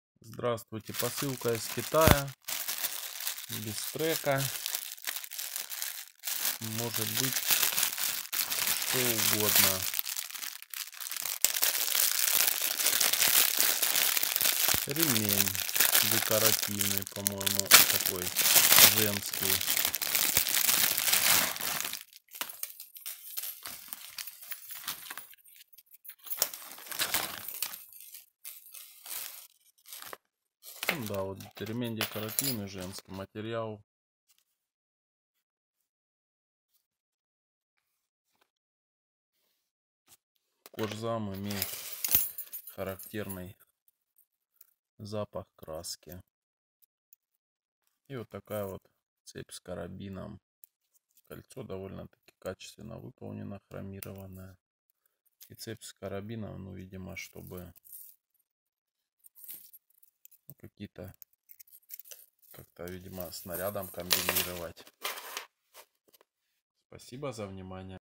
Женский ремень из искусственной кожи c aliexpress 06.01.2020
Снято на Sharp AQUOS S3 4/64